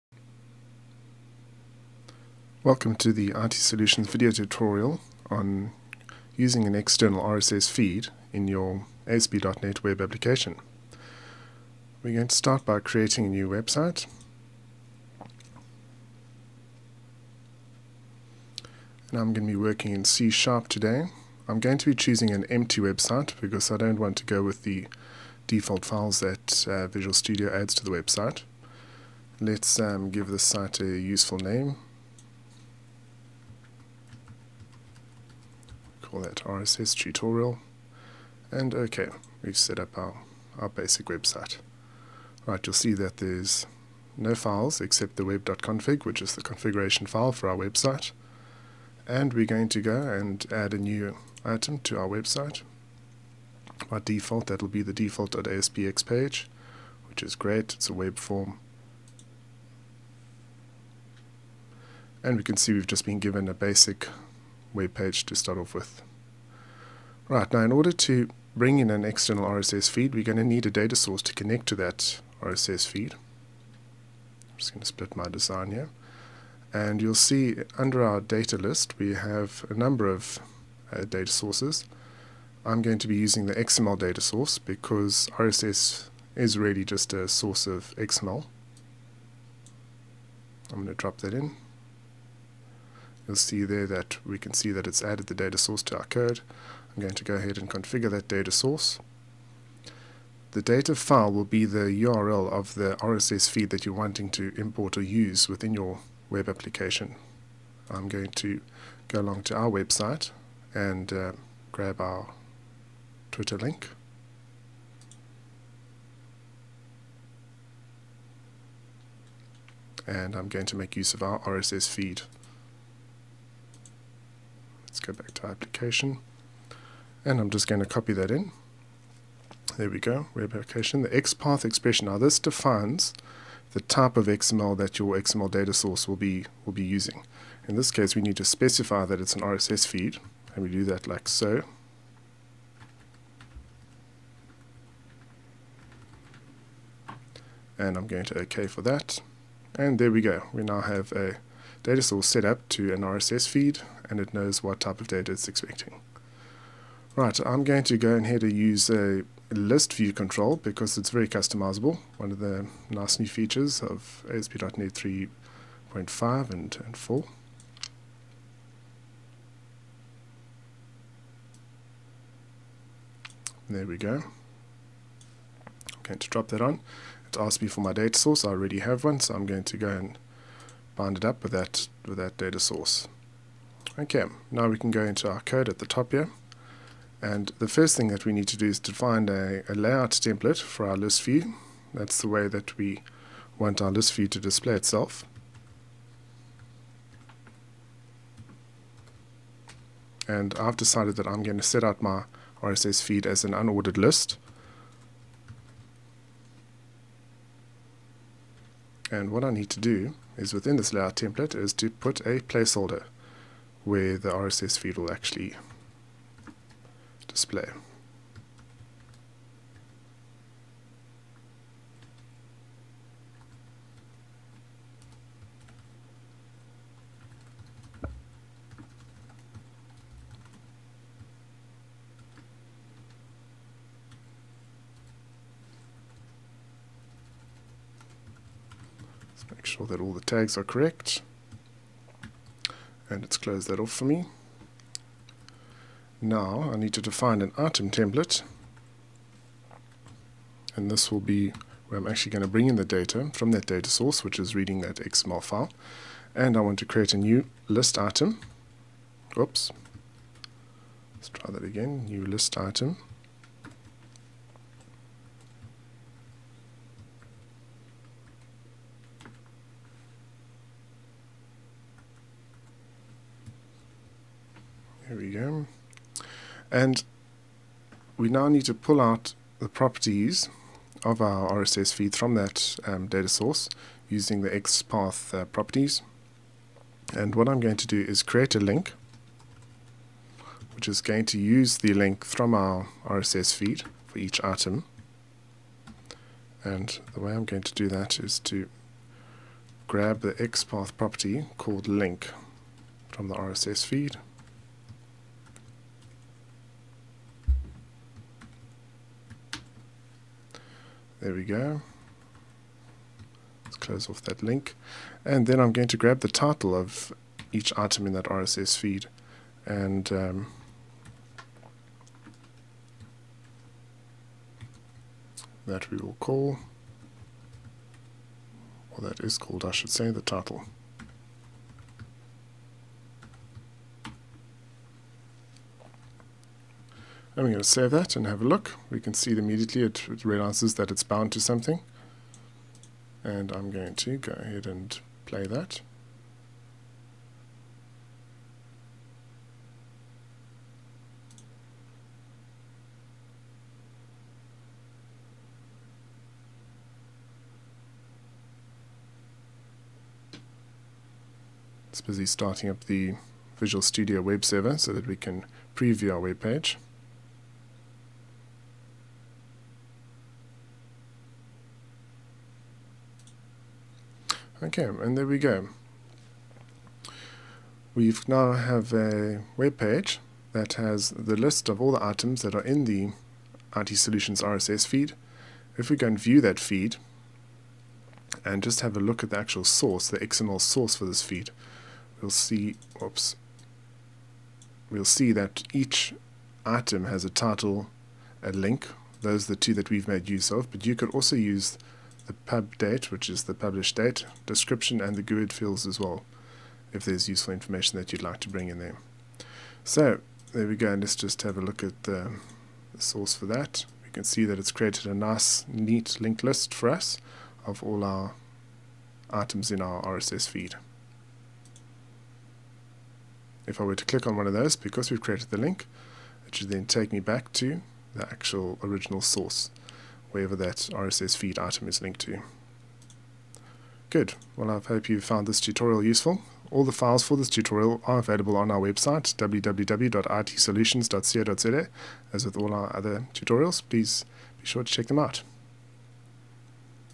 Use external RSS feed in ASP.NET website - Visual Studio 2010
How to consume an external RSS to manipulate and display in your ASP.NET website using Microsoft Visual Studio 2010.